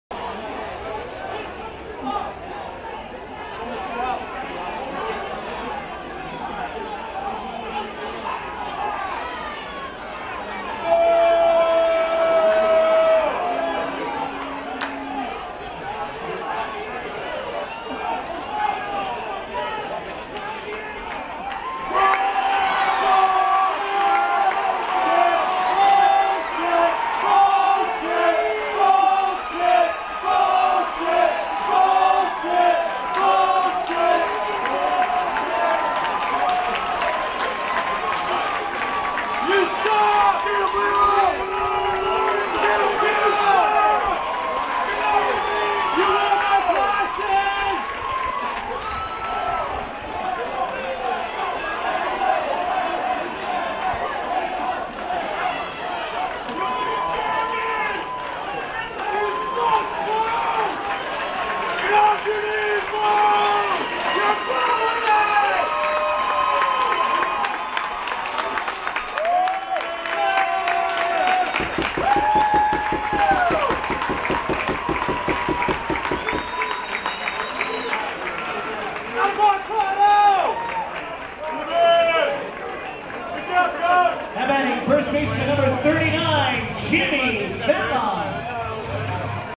Ejection video: Lelo Prado, USF Bulls
UCF Knights @ USF Bulls
USF Baseball Stadium